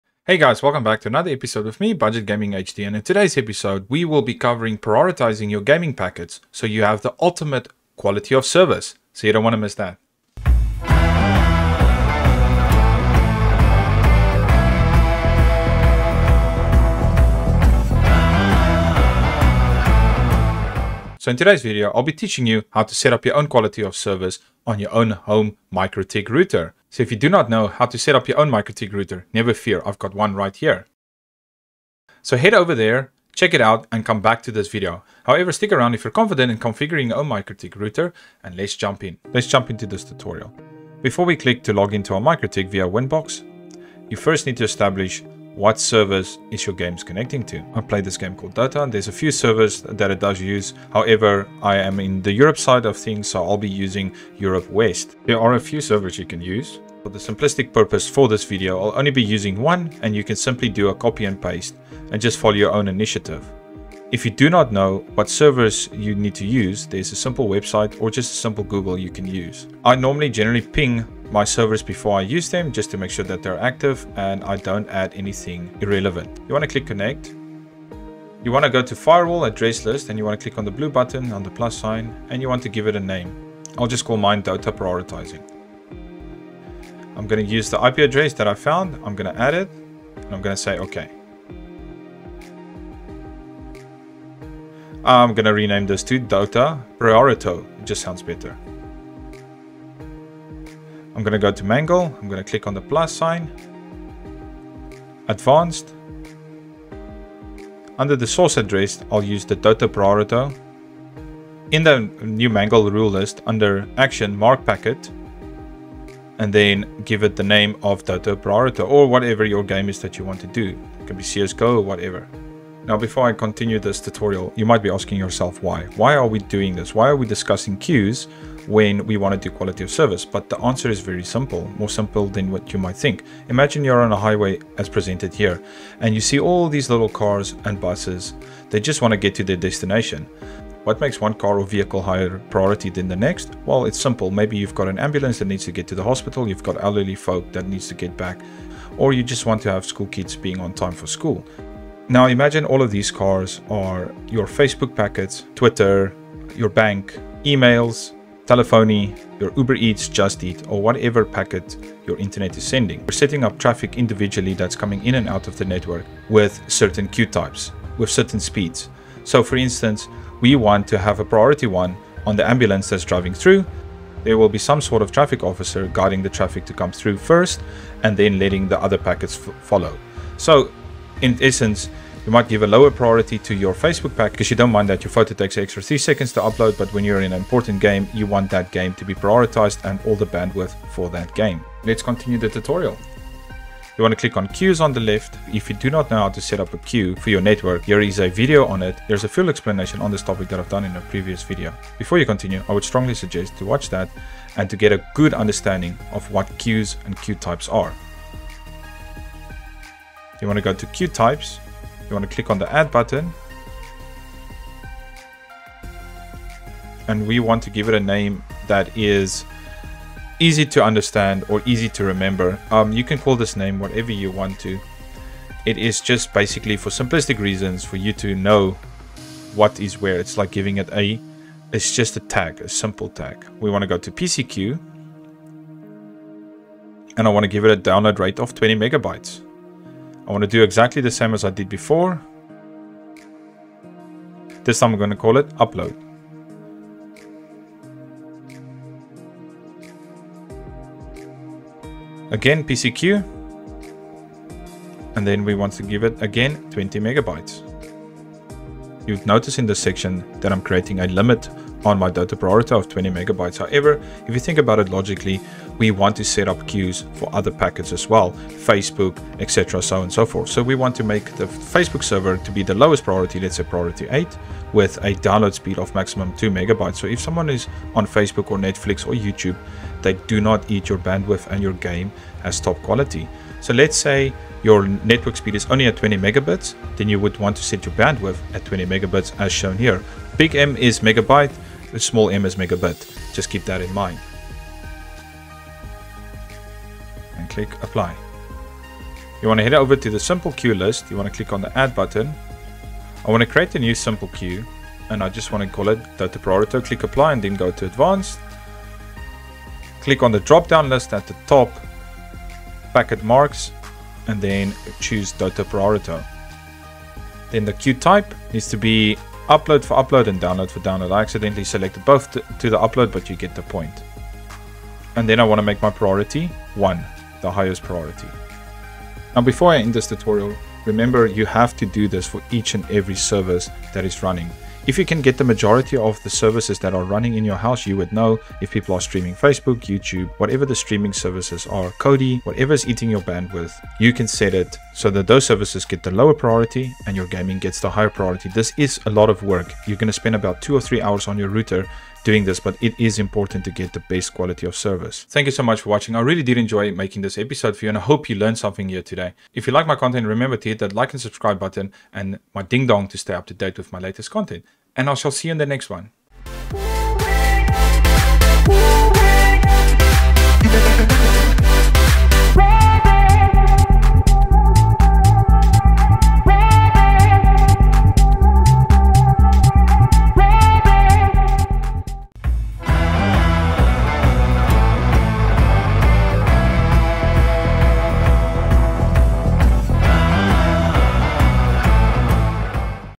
Quality of service for ultimate gaming on your own router! Goodbye Lag! Mikrotik router
00:00 - Intro
00:40 - Establish the servers
01:18 - Configuration
02:05 - Explanation of the why?
03:12 - More Configs
05:55 - Outro
06:06 - I am good at dancing